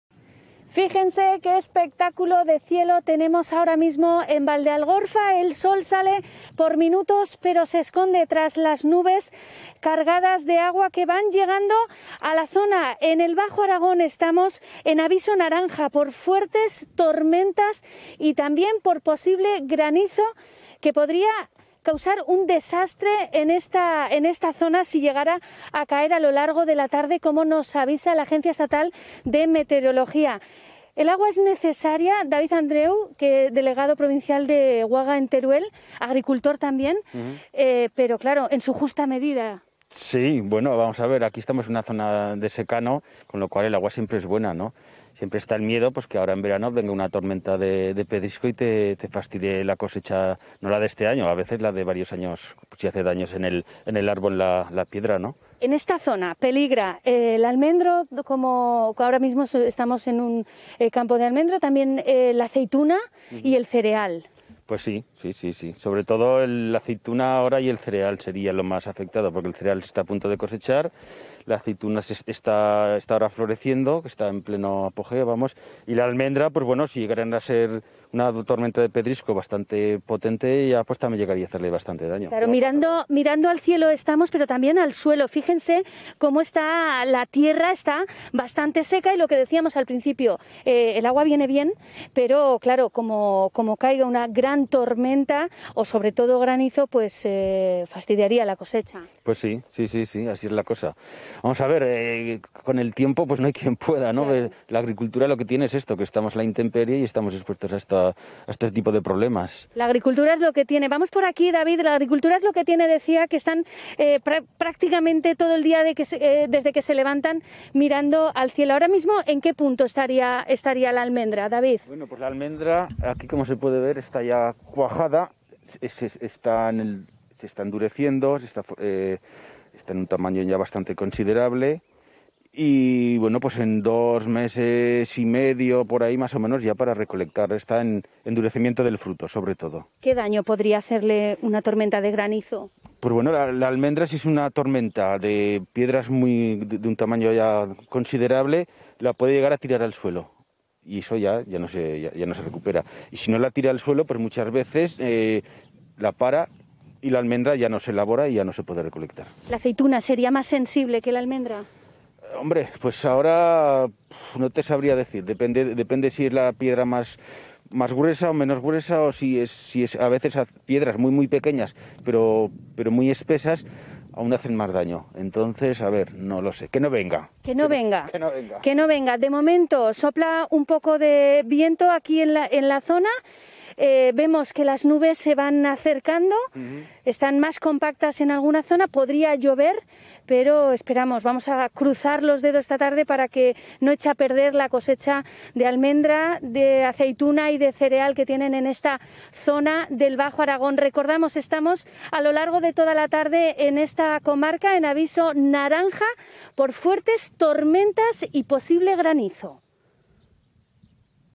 Peligra la cosecha de aceituna y almendra en Valdealgorfa por las tormentas
La alerta por tormentas amenaza las cosechas de aceituna y almendra en Valdealgorfa. David Andréu, delegado provincial de UAGA en Teruel nos explica los efectos que una tormenta de granizo tendría sobre las cosechas de la zona.